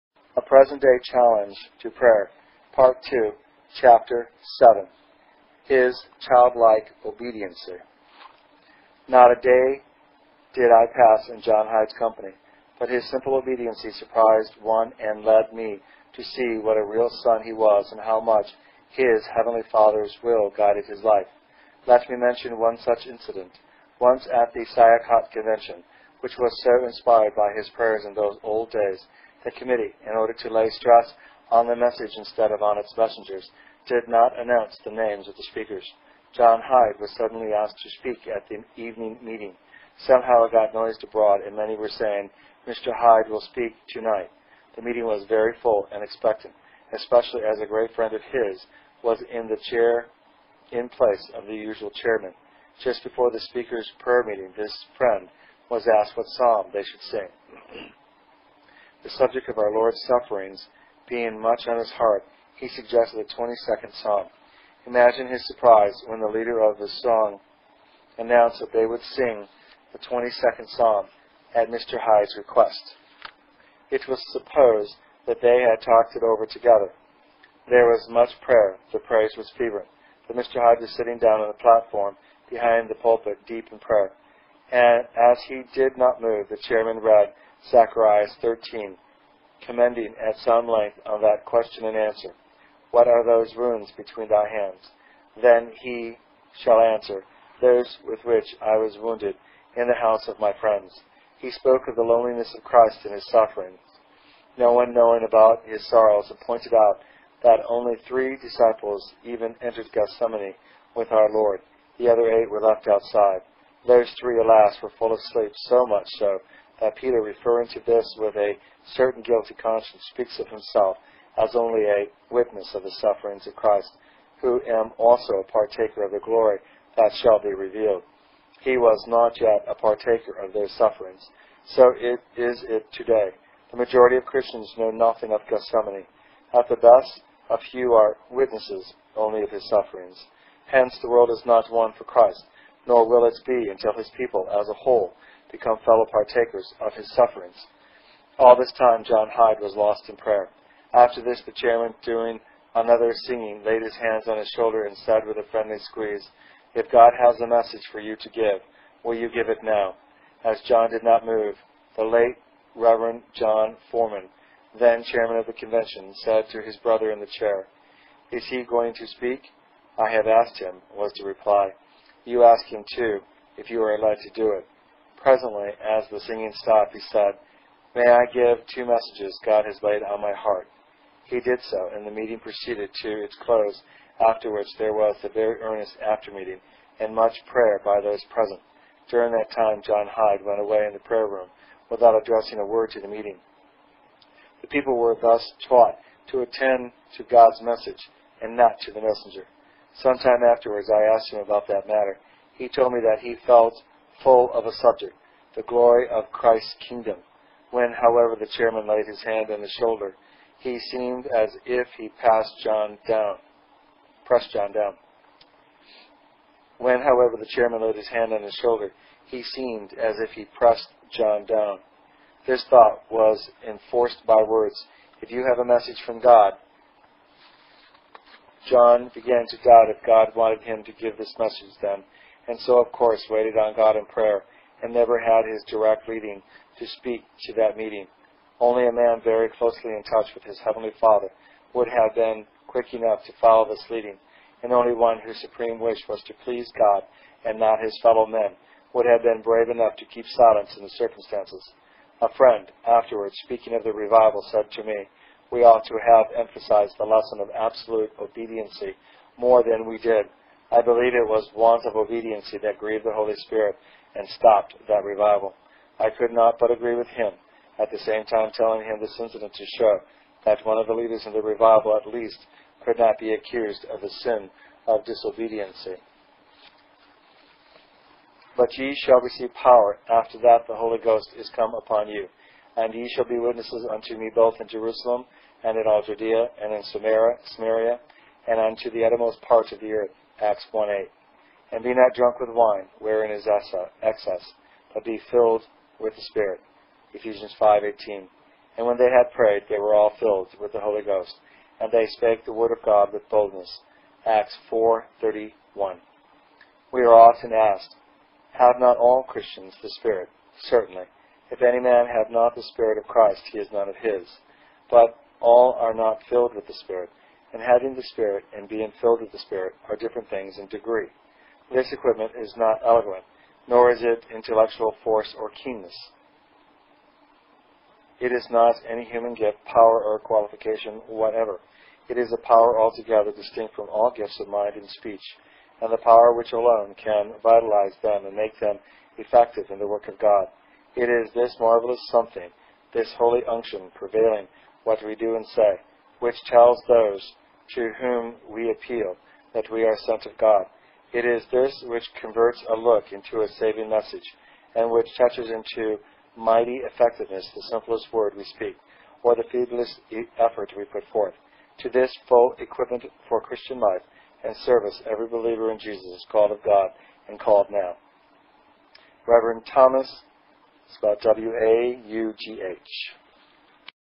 A Present Day Challenge In Prayer - Praying John Hyde - Section 2 Chapter 7
A Present Day Challenge In Prayer - Praying John Hyde by Captain E. G. Carre - Section 2 Chapter 7 SUPPORT OUR MINISTRY BY CLICKING ON ALL THE ADS POSSIBLE PLEASE. this way we are paid some money from the Advertizers.